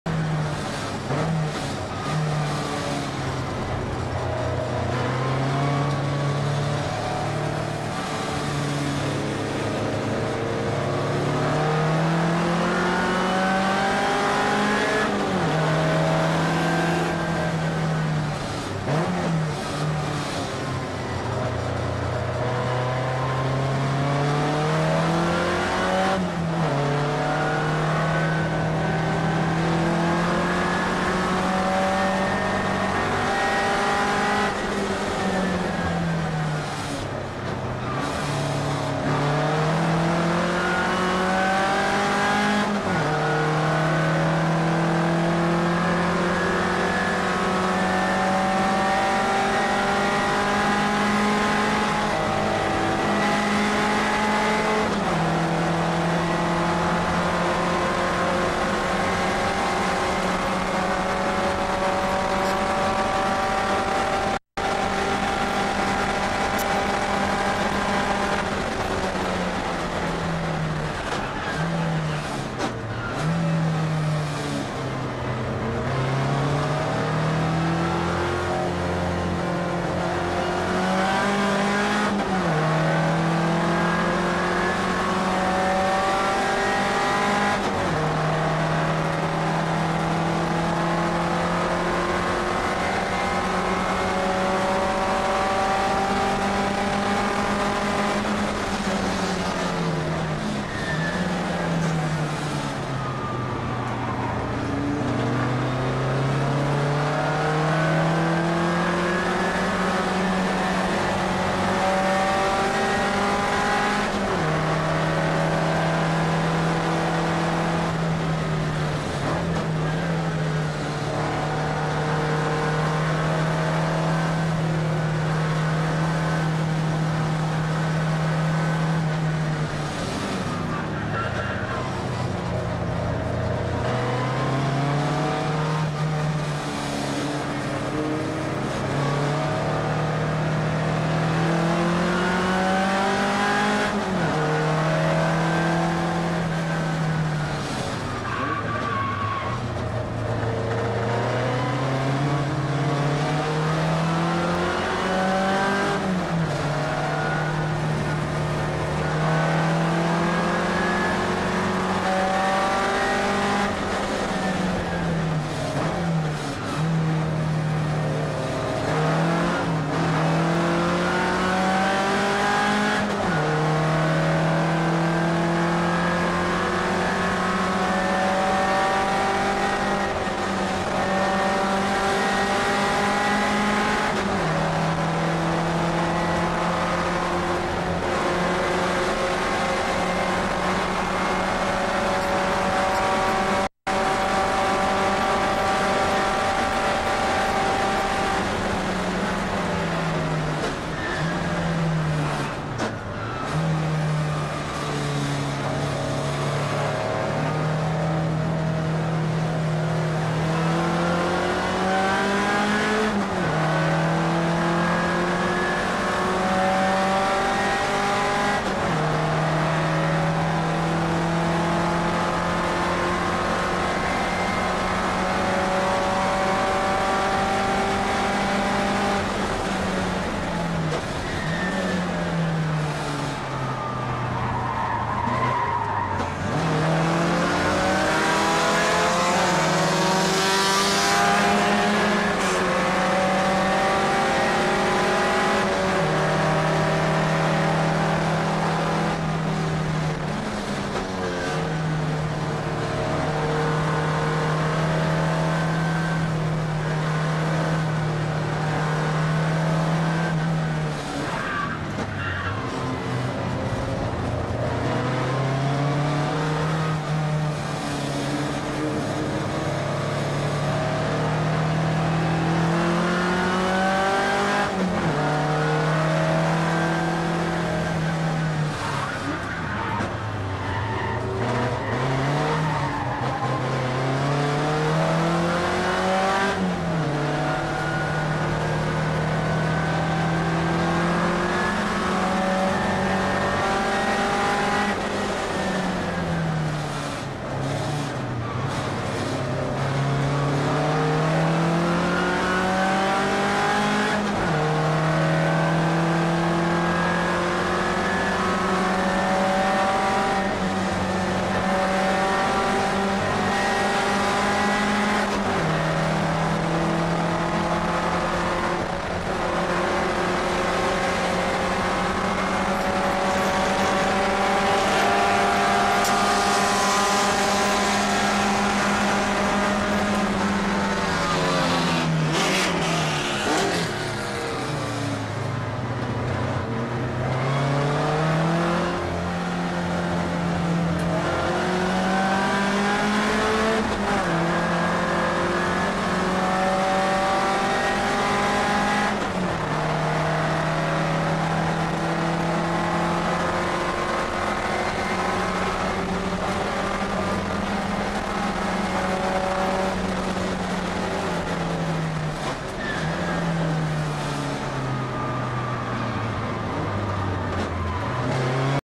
10a etapa SuperClassic 2008 - Disputa Fusca 914 Paulo Sousa com Passat Fabio Fukuda - Interlagos
Domingo, Dez-14, 10a etapa SuperClassic 2008. Disputa entre Fusca AP no.914 e Passat do Fukuda.